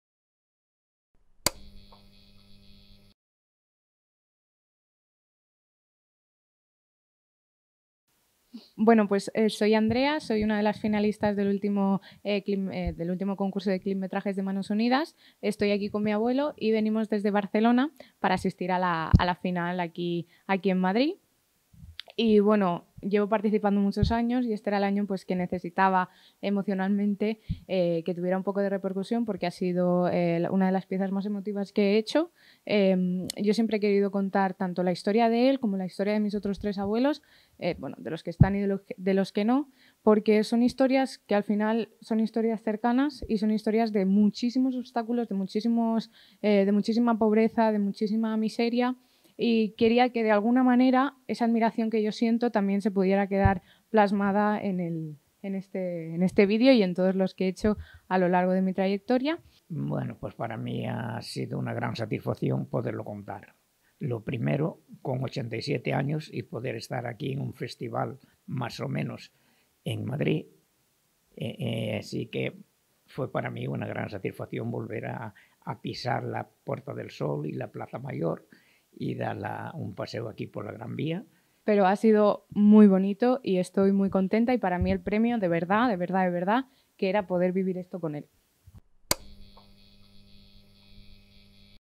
Andrea Rodríguez. Finalista. Festival de Clipmetrajes XIII ed.
En esta entrevista, Andrea Rodríguez, nos habla de su experiencia como participante en esta XIII edición del Festival y sobre lo que siente al haber sido su clipmetraje uno de los diez mejor valorados por el jurado.